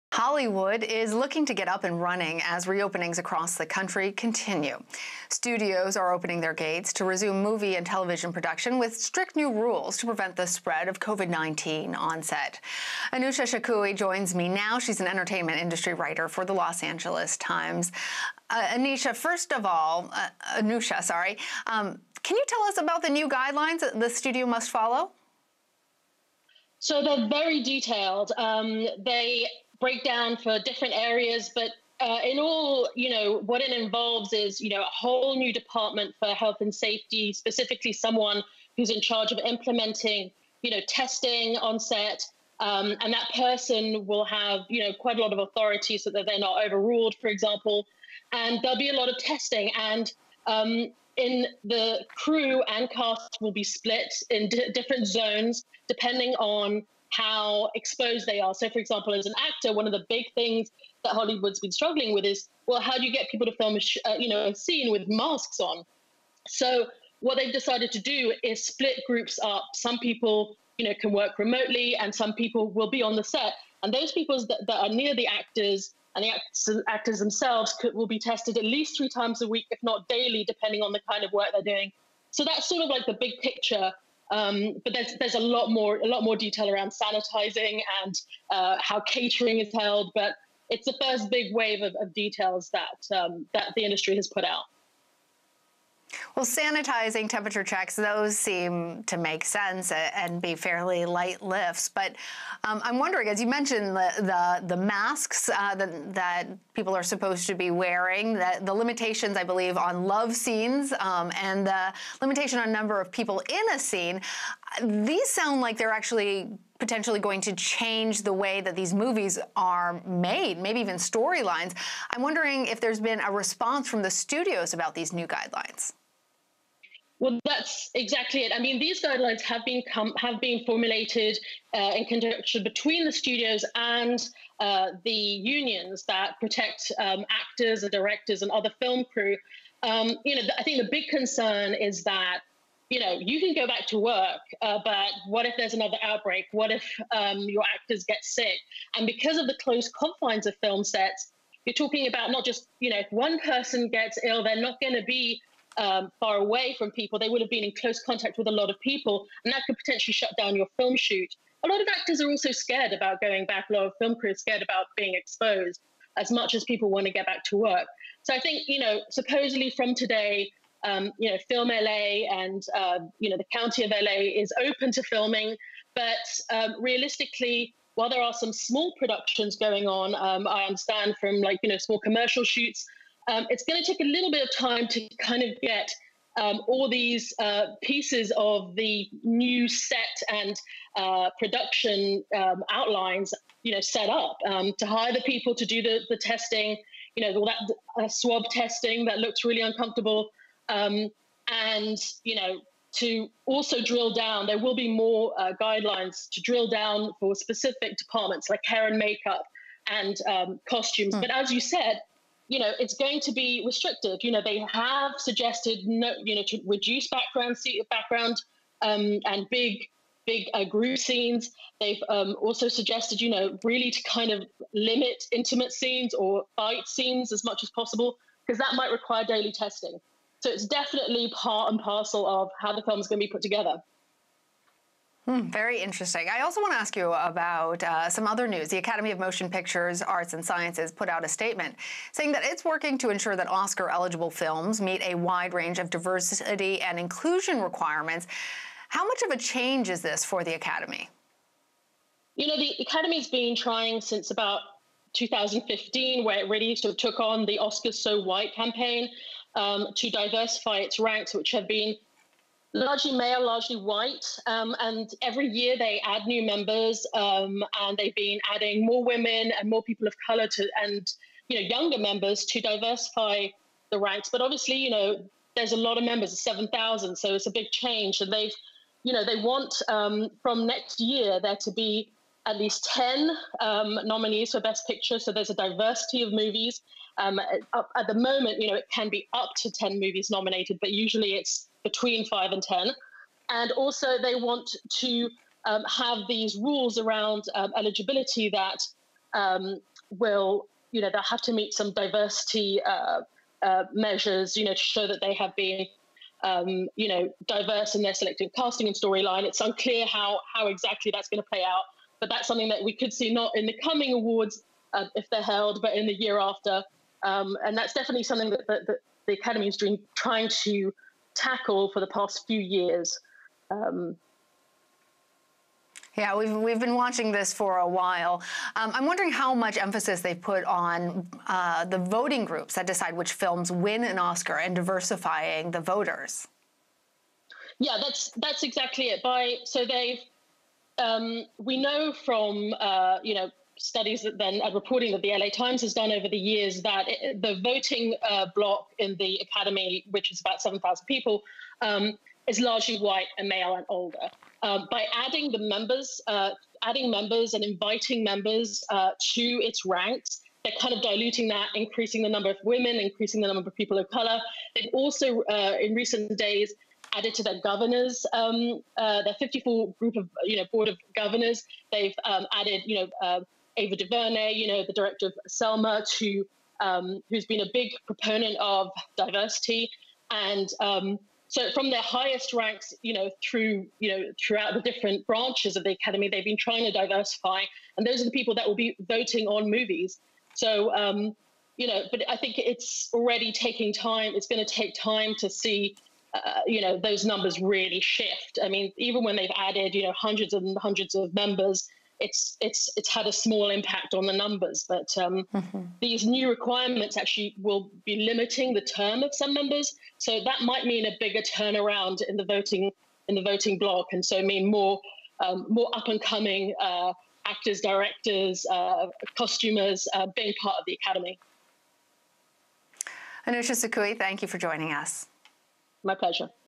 Hollywood reopens for production as Academy boosts diversity efforts
As movie studios reopen, there will be strict guidelines for film production to prevent the spread of COVID-19. This comes as the Academy of Motion Picture Arts and Sciences announces a new initiative to ensure that Oscar-eligible movies meet diversity and inclusion requirements. Anousha Sakoui, an entertainment industry reporter for the LA Times, joins CBSN to discuss.